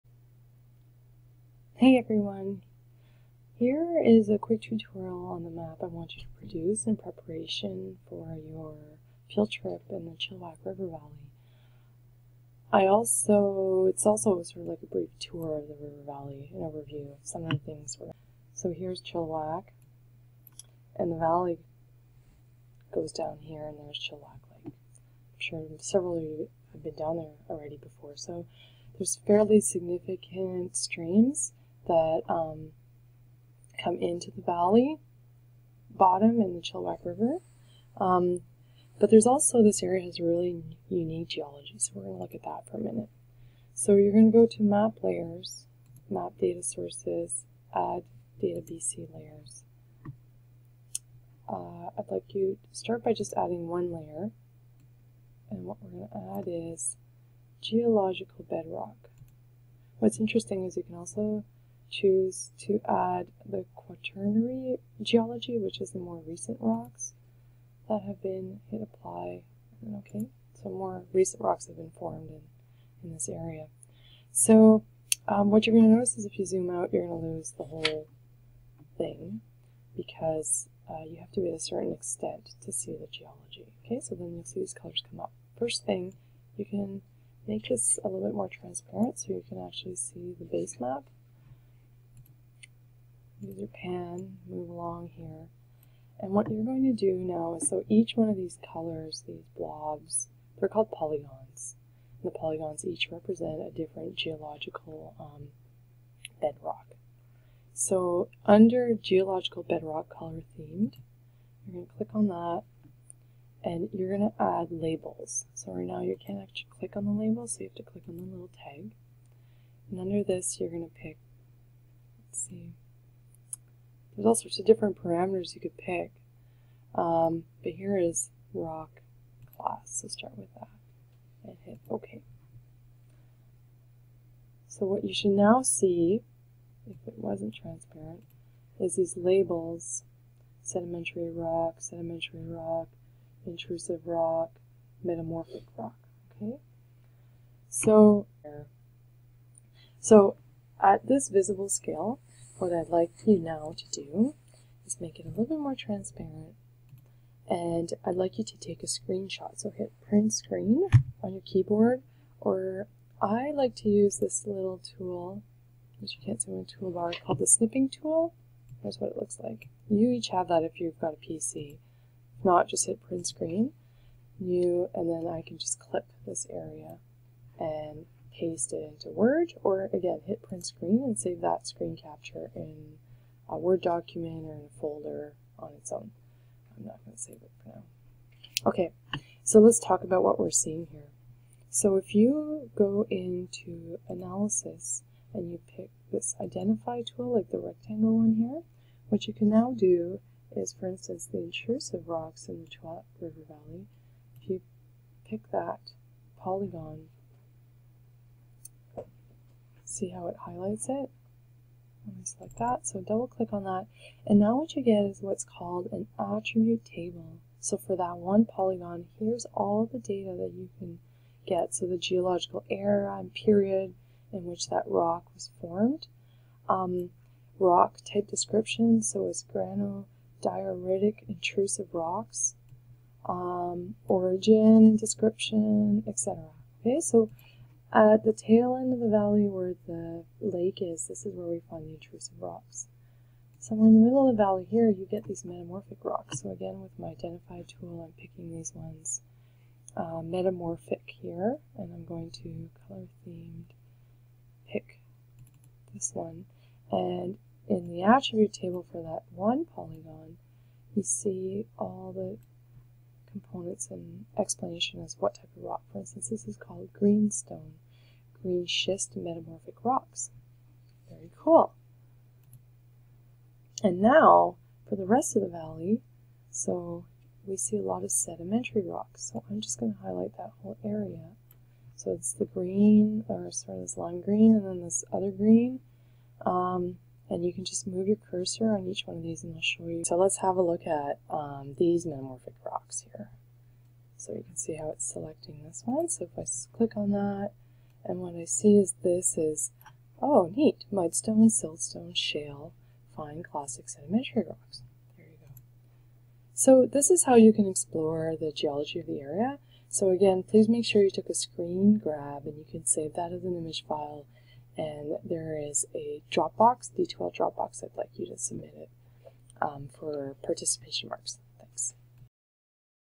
Chilliwack River Valley Geology Map
How to use iMap to produce a geology map of the valley.